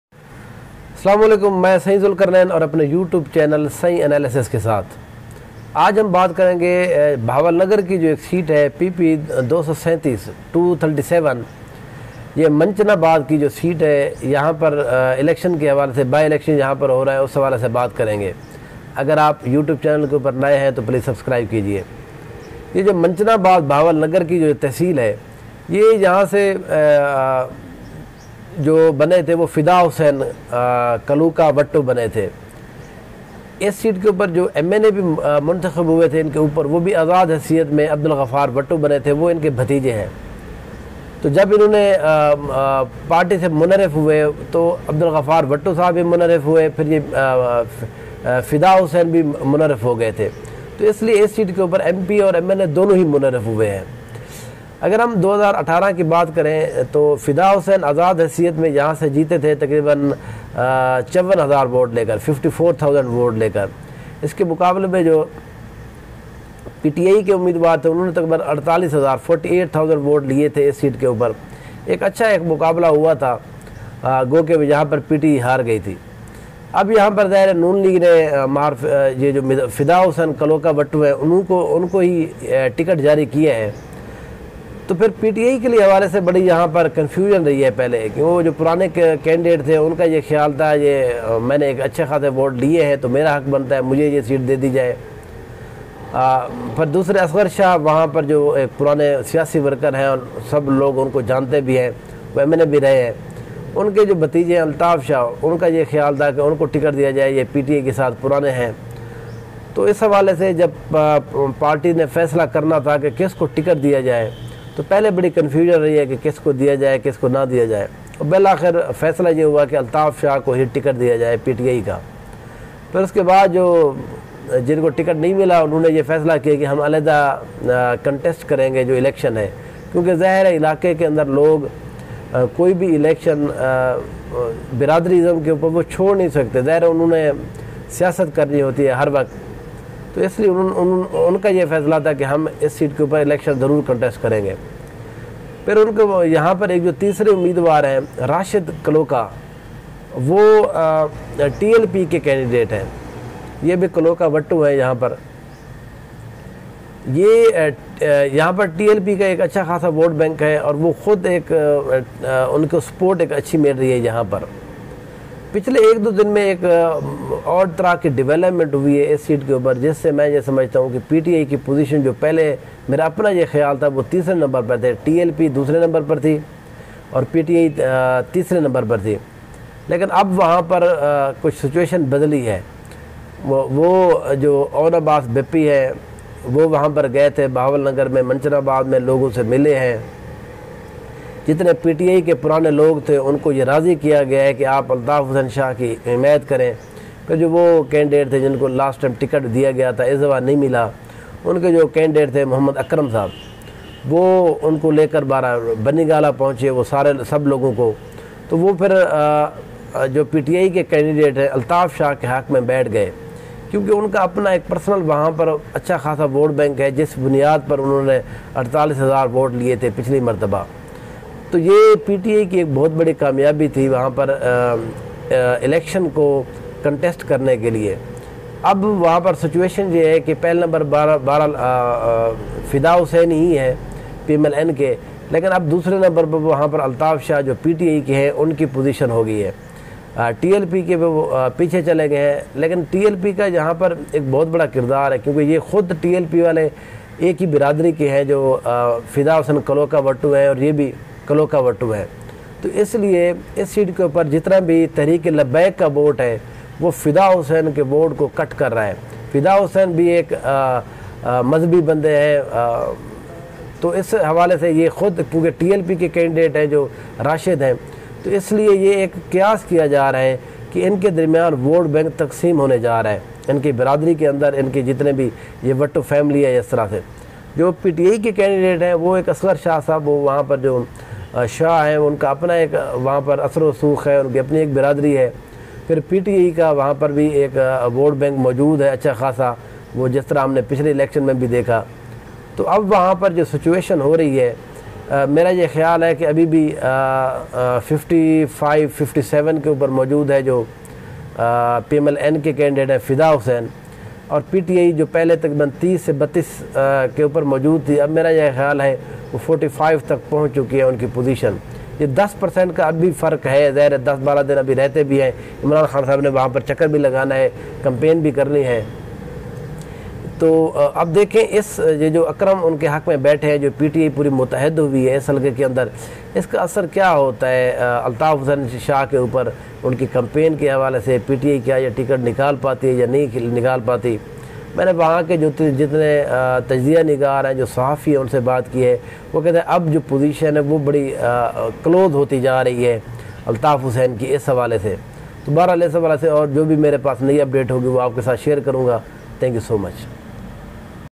PP 237 Bahawalnagar by election: Sain's Analysis: Wattoo vs Wattoo vs Shah: PTI vs PMLN vs TLP
Fida Hussain Wattoo is a strong candidate who joined PMLN. In 2018 general elections, Fida Hussain won as Independent candidate.
PTI got almost 4800 votes in 2018 general elections, so PTI have their vote bank there.
TLP is also going well on this seat.